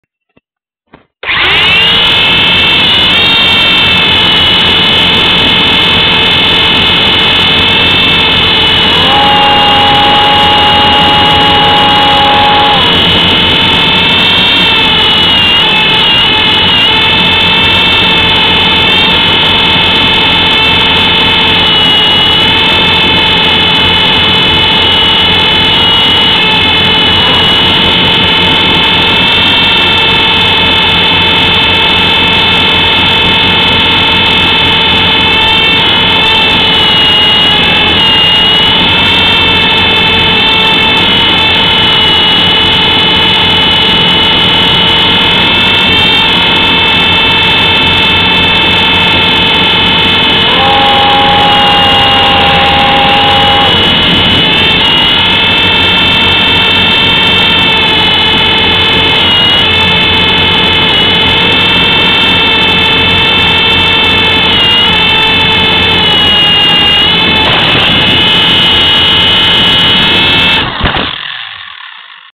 Heli_2.mov
Rundflug mit Micro-Heli und Keychain Cam